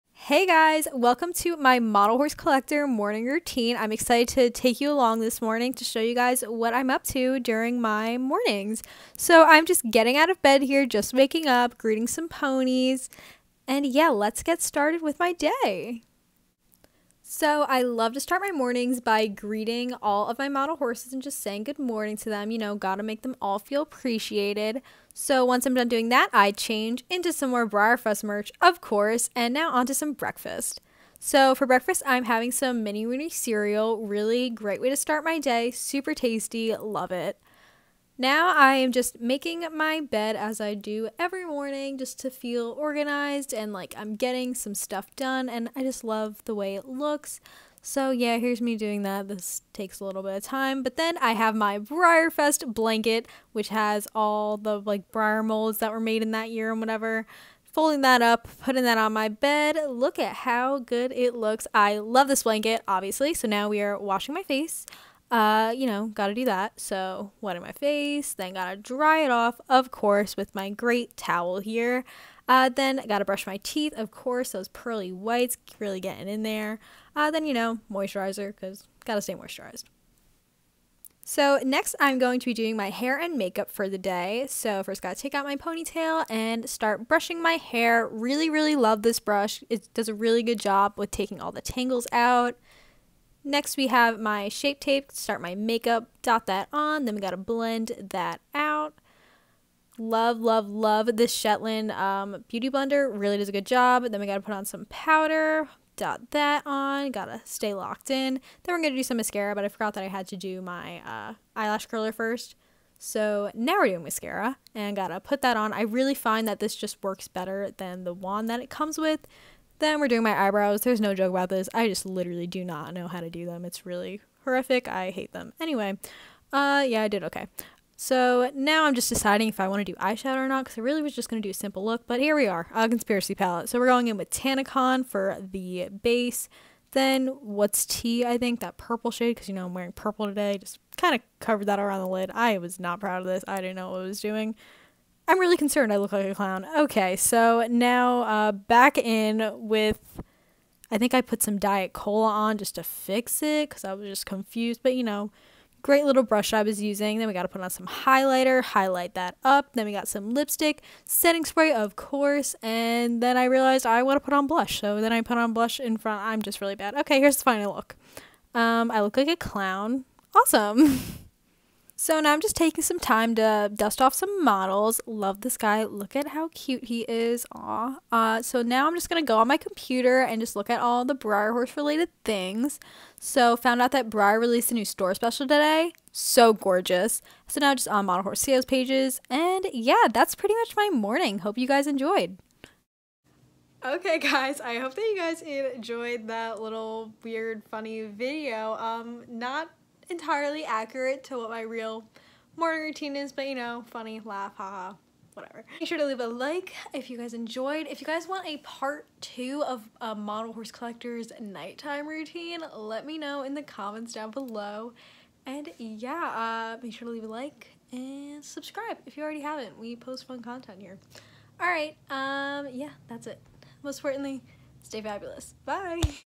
Model Horse Collector Morning Routine
hey! hope you enjoyed this little video about your average morning as a model horse collector lol
stay fabulous